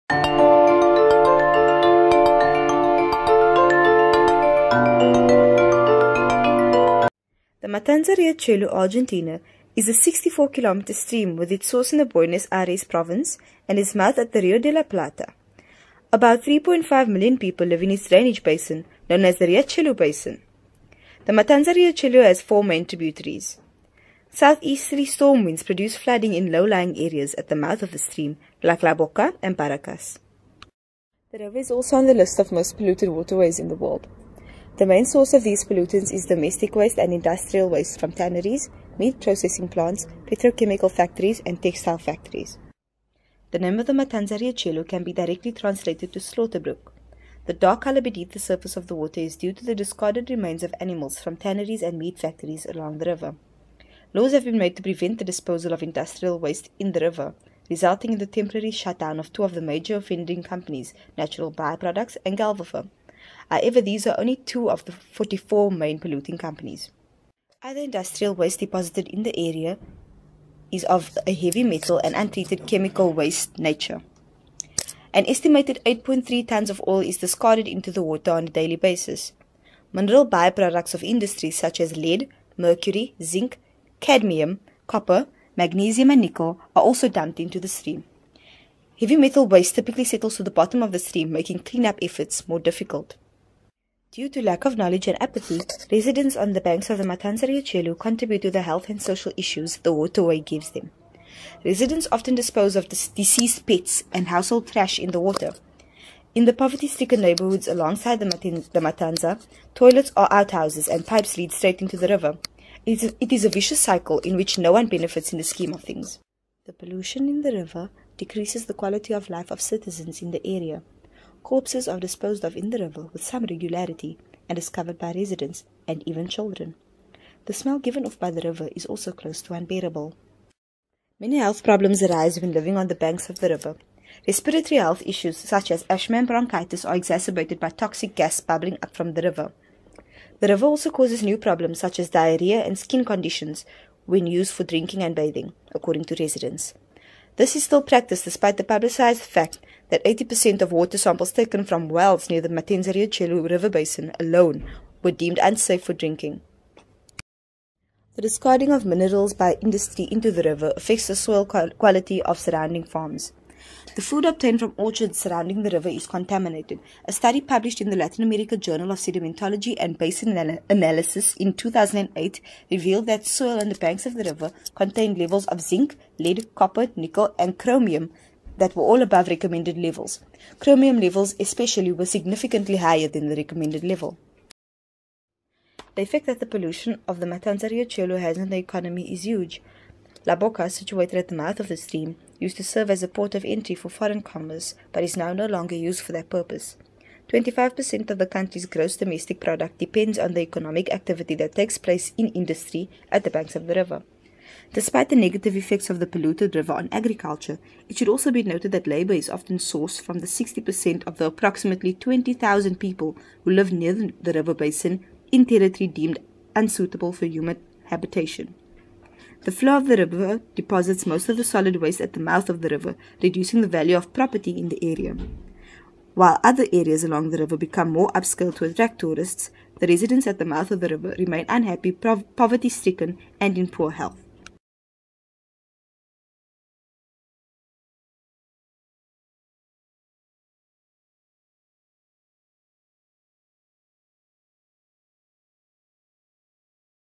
Economic impacts of pollution in Matanza-Riachuelo
A description of the economic impacts of pollution in the Matanza-Riachuelo.

Made By Kim Daniels
Produced By Qarbon Qampus
Share Alike
No Derivatives
Non commercial
Attribution provided

3. Glenn T (2014). Transboundary Water Resources. Mantanza-Riachuelo river basin: A case study in watershed governance.